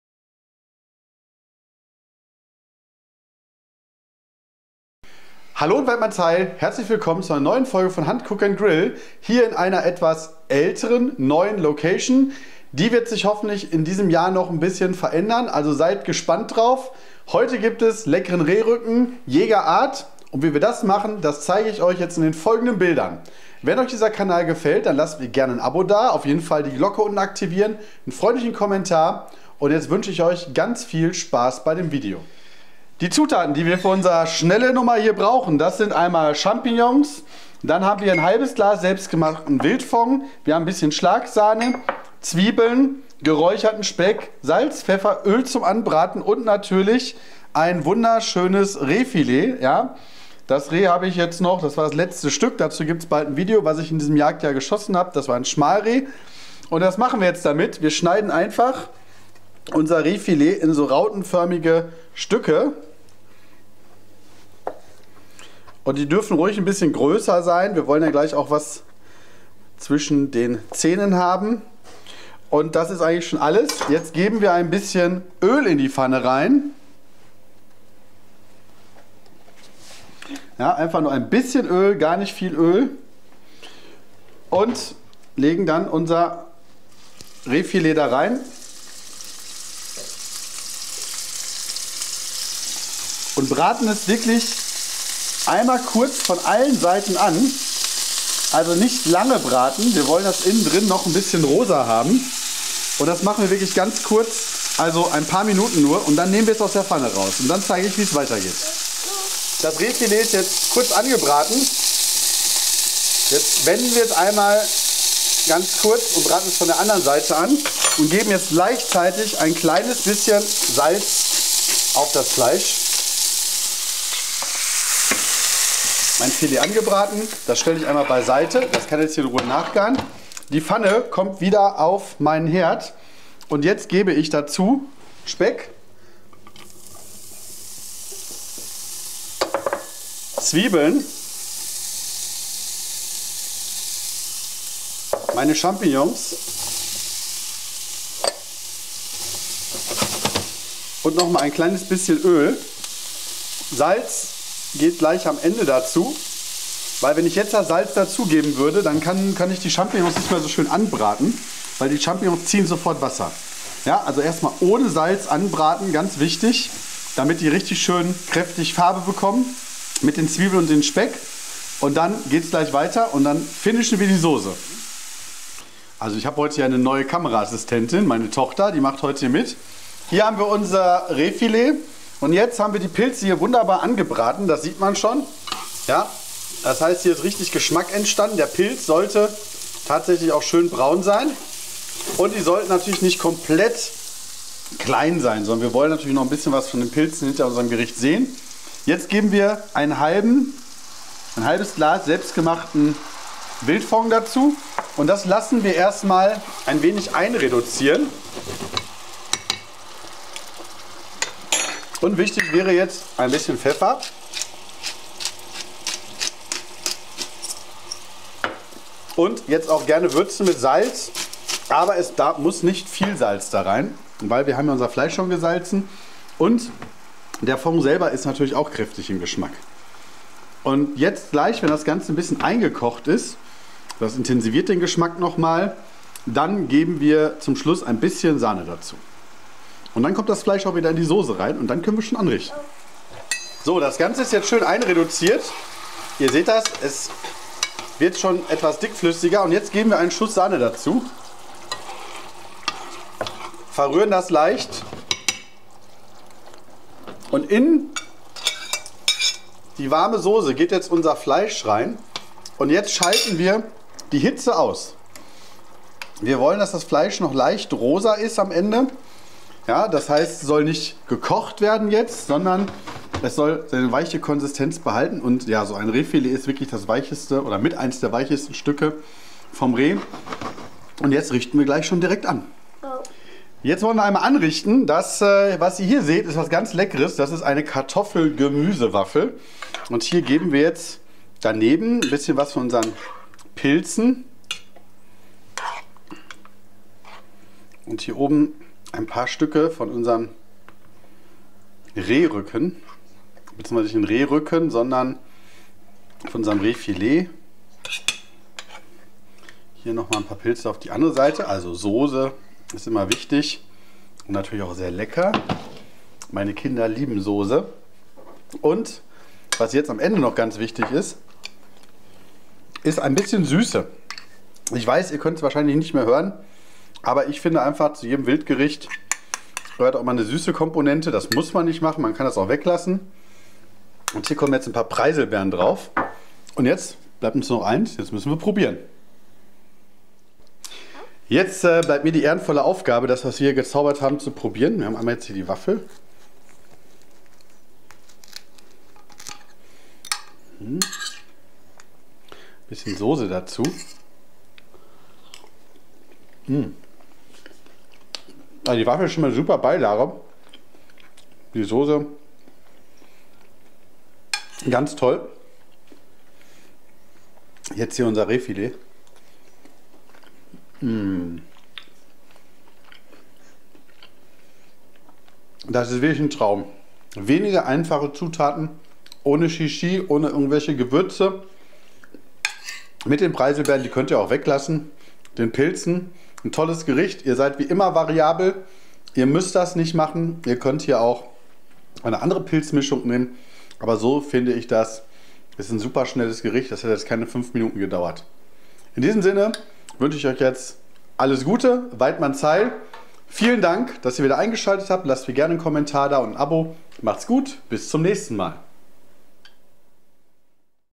SCHNELLENUMMER - Rehfilet Jäger Art
In diesem Video gibt es ein schnelles Rezept mit Filet vom Reh!

Viel Spaß und Waidmannsheil